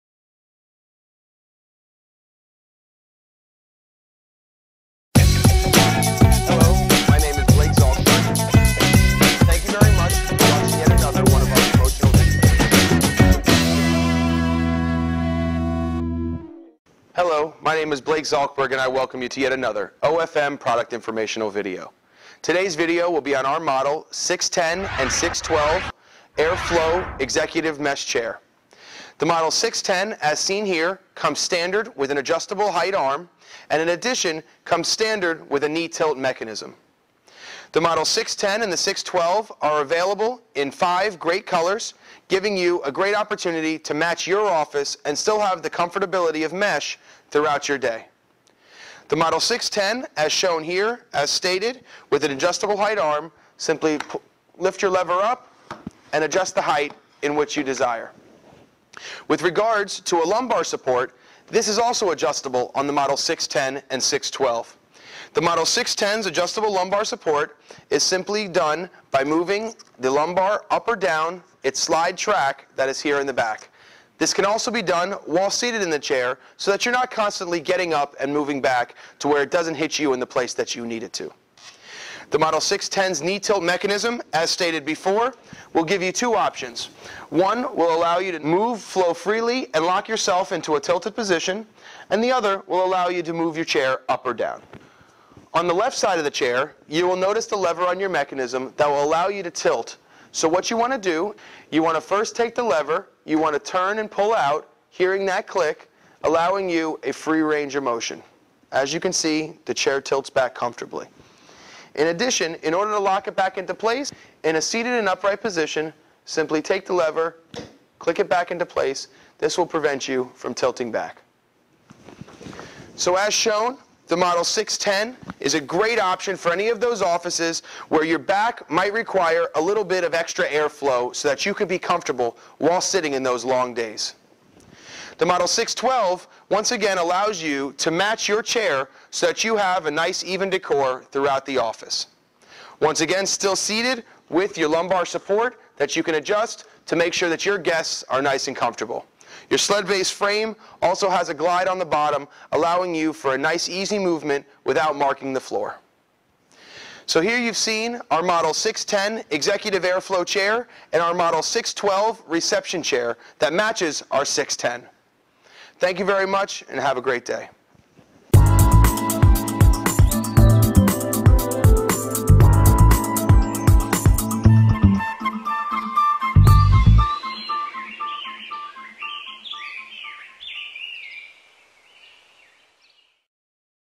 OFM Mesh Airflo Executive Chair 610
The AirFlo chair features a designer-style mesh back that feels as great as it looks! Helps you "keep your cool" all day! The Sleek, modern design features a hi-tech mesh back for excellent ventilation. The tilt-lock and adjustable lumbar support provide superior fatigue-fighting comfort.